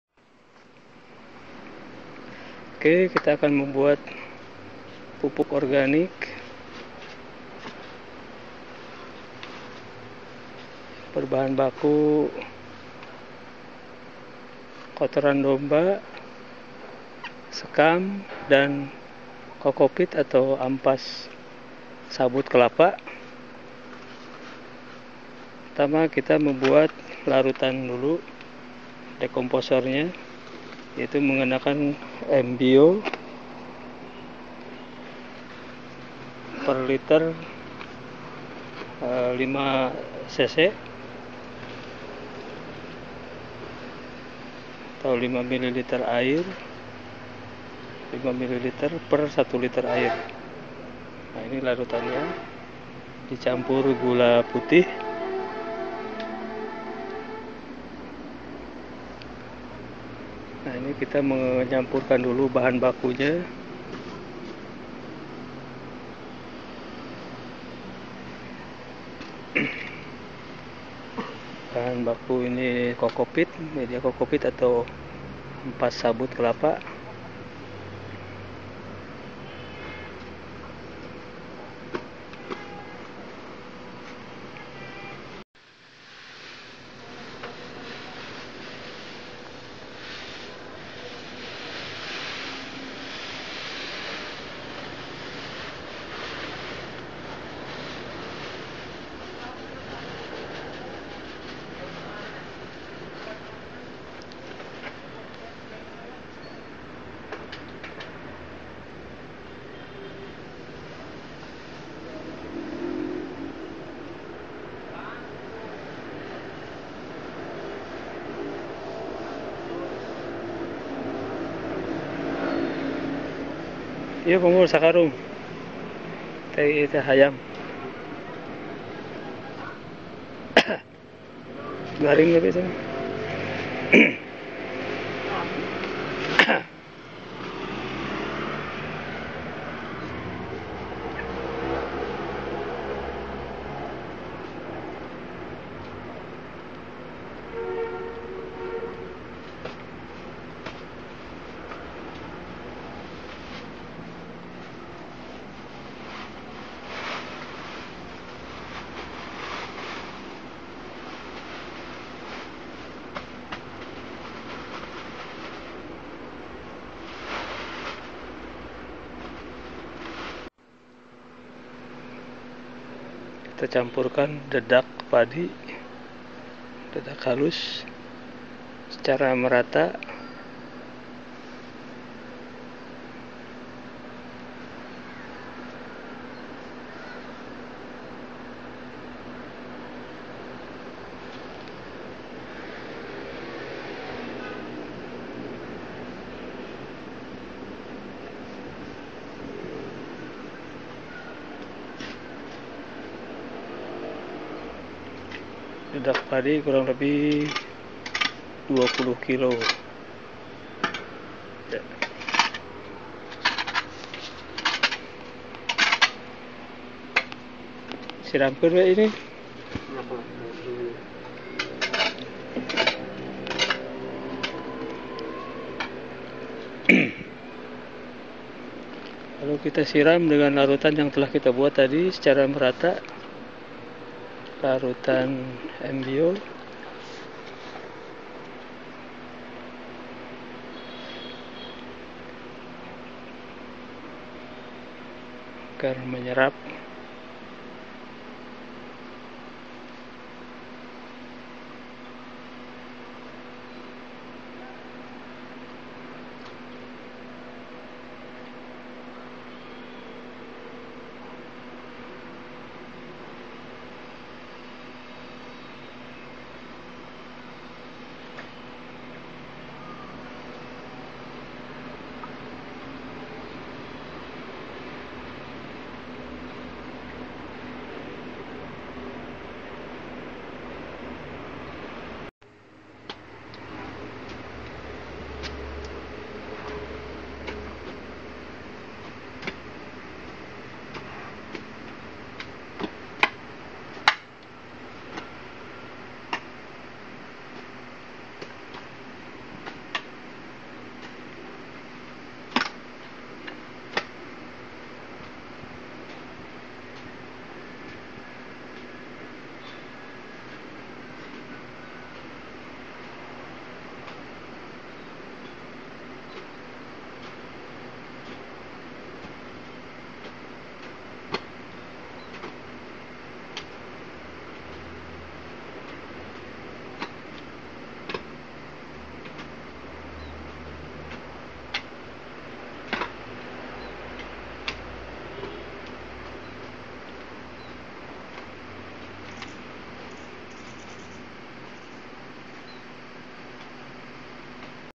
Membuat Pupuk Organik - PORASI
Cara Membuat Pupuk Organik PORASI atau Pupuk Organik Hasil Fermentasi, dengan Bahan Baku Sebagai Berikut :
- Kotoran Domba / Kambing
- Cocofeat / Ampas sabut kelapa
- Sekam Padi
- Arang Sekam
- Biofertilizer M-Bio
- Gula Pasir
- Dedak Padi

Lama Proses pembuatan PORASI biasanya selama 7 hari